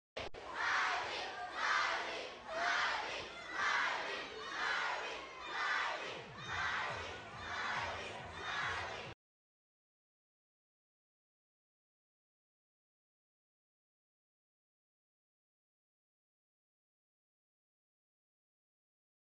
Miley Cyrus Thinks her Fan is hot at her signing session!!
it's theirs not mine :')
LAWL.